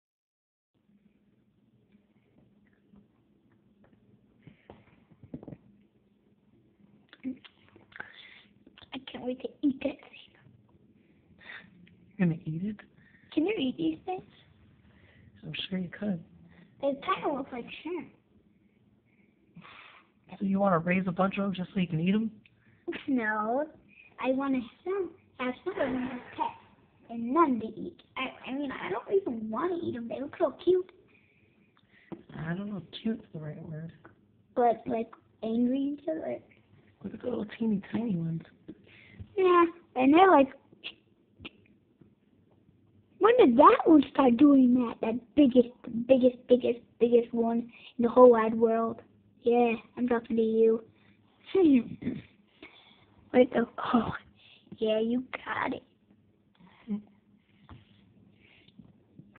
My Triops =)
From December 26th til today (the 11th of January) this is what they look like.. all different sizes although I put the eggs in the water at the same time. Neat! Can't wait to transport them! 3